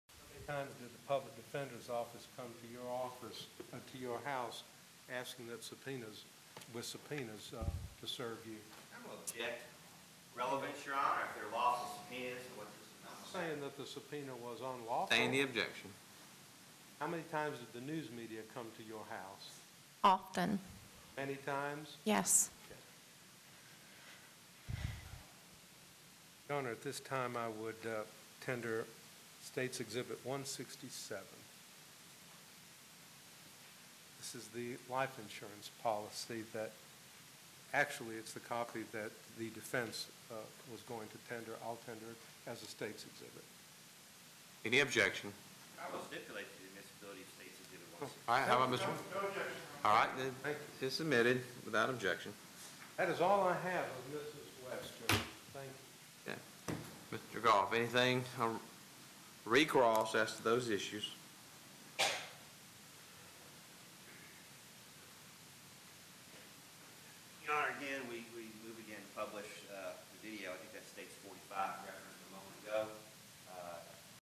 GA:BABY MURDER TRIAL MOM DEFENDS HER VERSION
Sherry West defended her recollection of how her 13-month-old child was killed in Brunswick GA as she testified Wednesday.